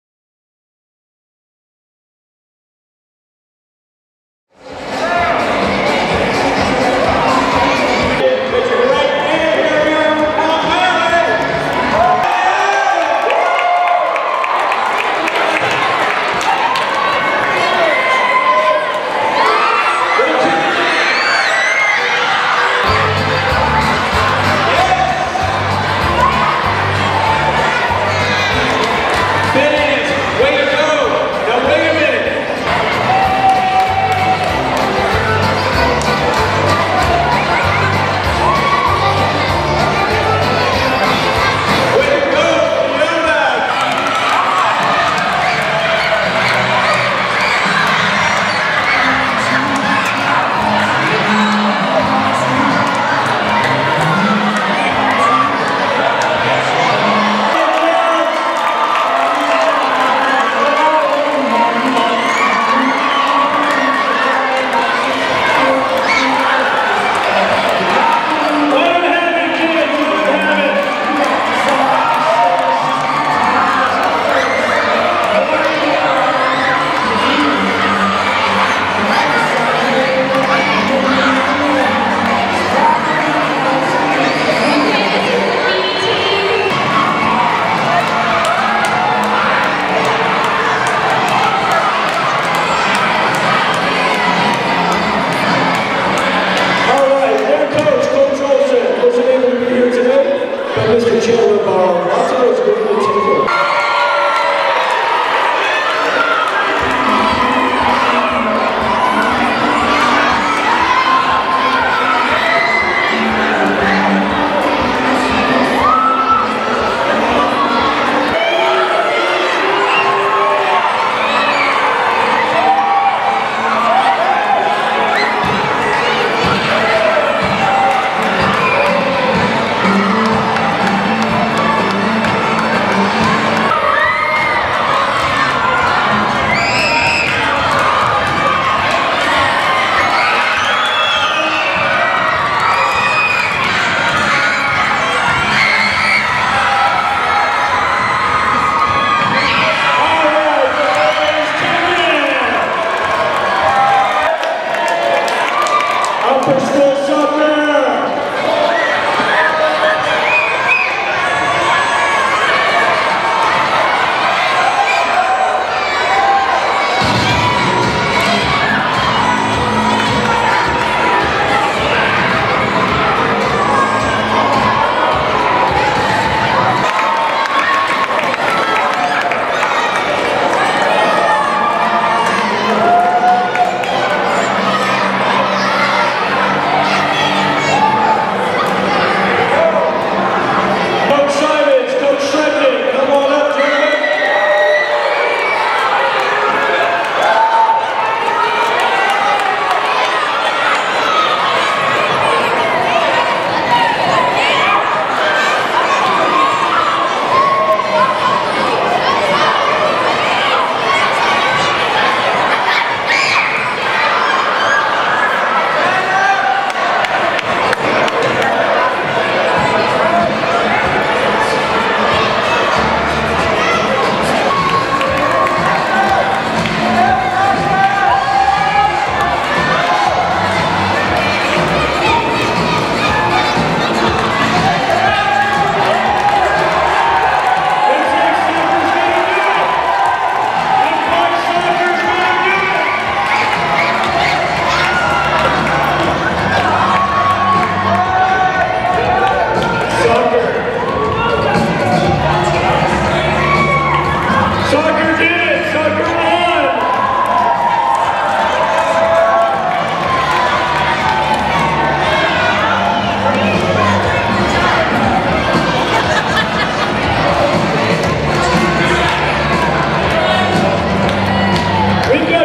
MWPV's Pep Rally Winter 2013
Marywood-Palm Valley School's 2013 Winter Pep Rally, featuring the Middle School Basketball teams and the Varsity Soccer and Varsity Boys and Girls Basketball teams.